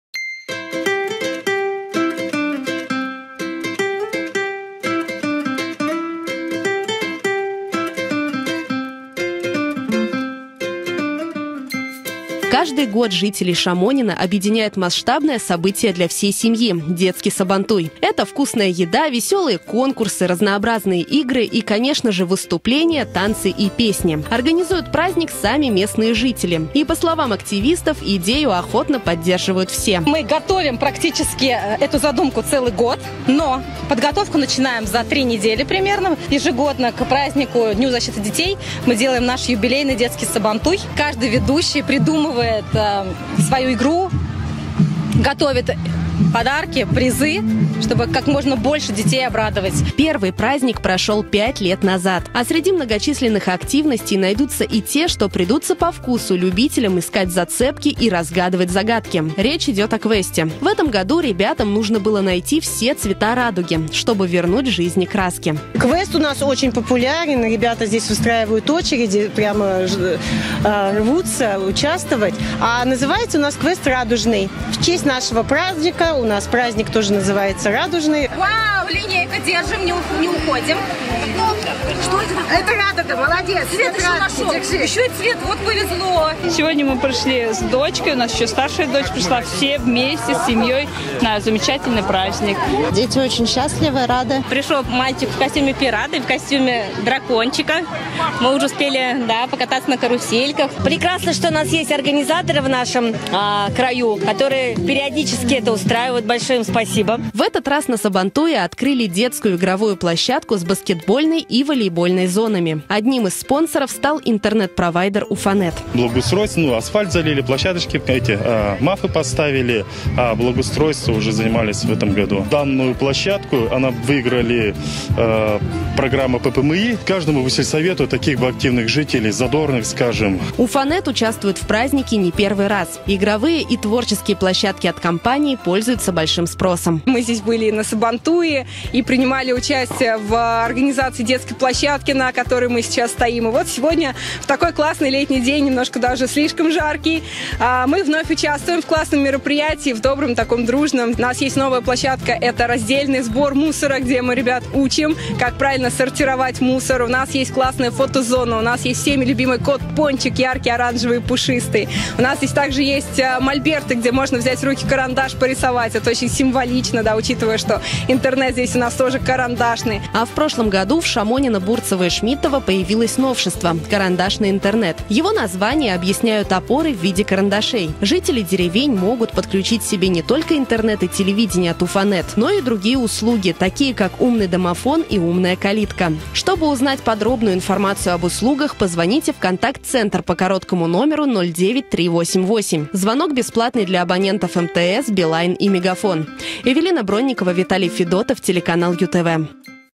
Под Уфой прошёл масштабный детский Сабантуй. Он объединил жителей трёх деревень
Жители Шамонино уже пятый год устраивают в своей деревне детский Сабантуй. К ним присоединяются жители Бурцево и Шмидтово: и дети, и взрослые находят себе занятие по душе. Это конкурсы, квесты, и выступления, а также игровые и творческие площадки от компании «Уфанет».